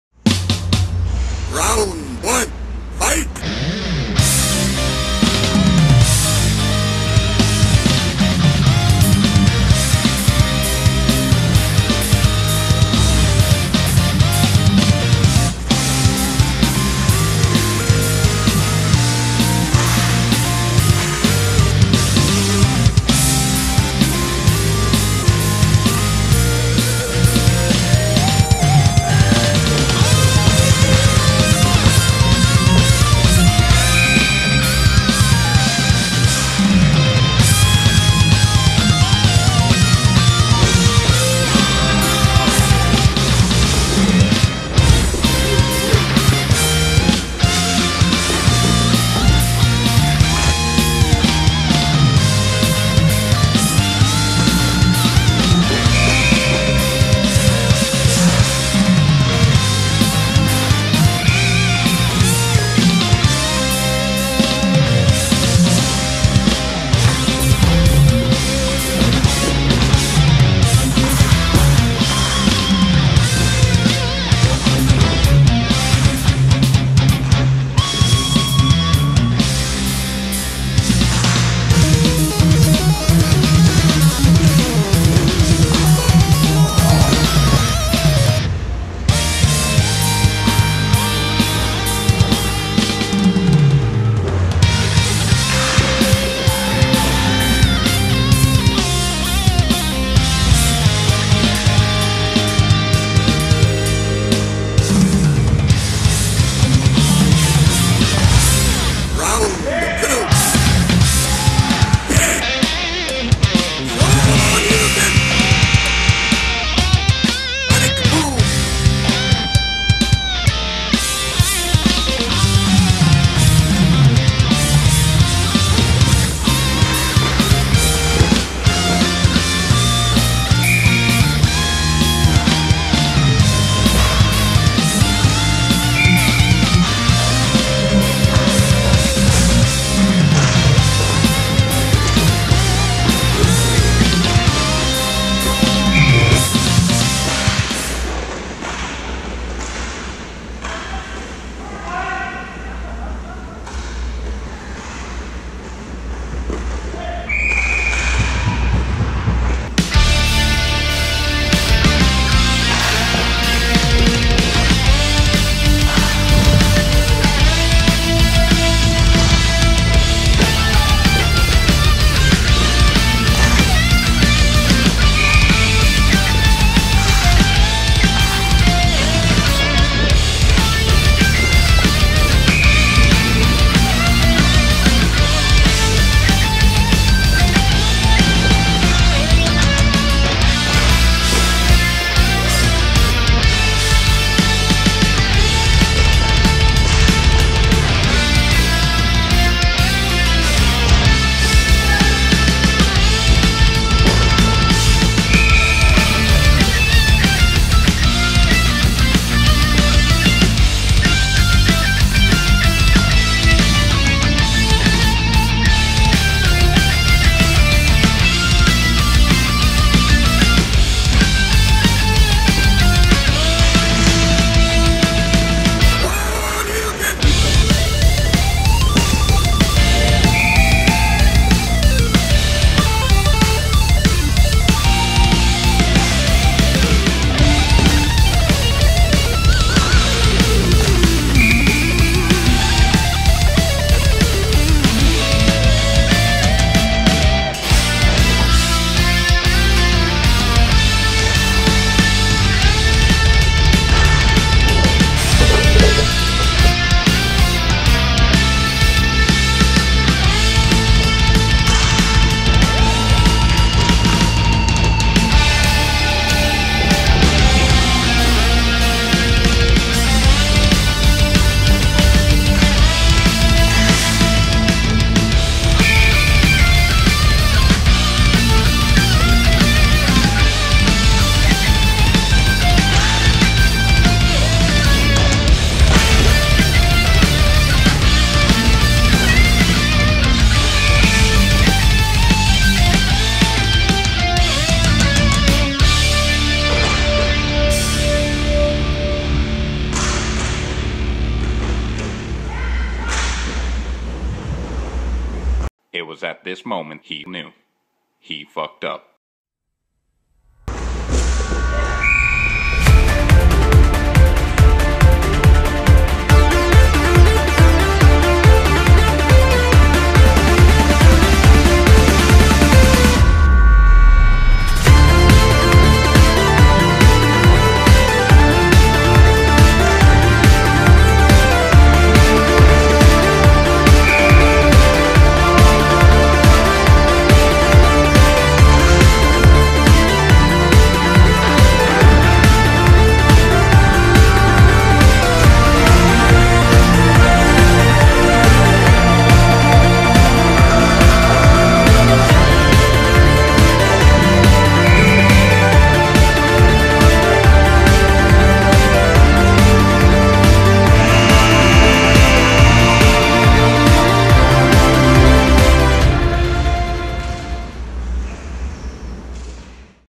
TimmyTheGoalie Season 6: Game 1 "'Ere We Go!"
A new season at the JCC puts my brother and I on team Dziuban facing team Rossi on 6/23/19

Breakfast Club Goals and saves highlights.